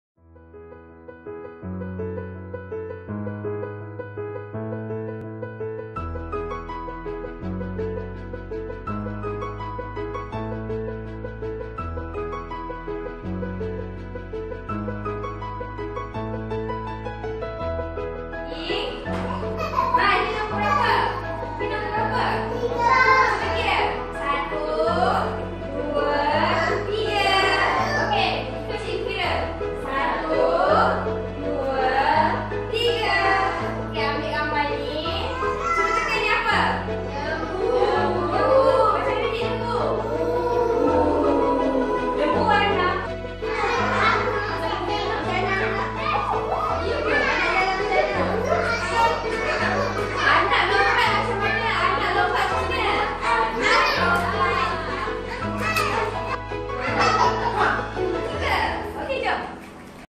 TASKA COMEL ANAKKU (AKTIVITI : ANIMALS MAP)
KELAS EPAL ( 2 TAHUN - 4 TAHUN)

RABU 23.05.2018

AKTIVITI : ANIMALS MAP

- Menguji tahap pemikiran logik kanak-kanak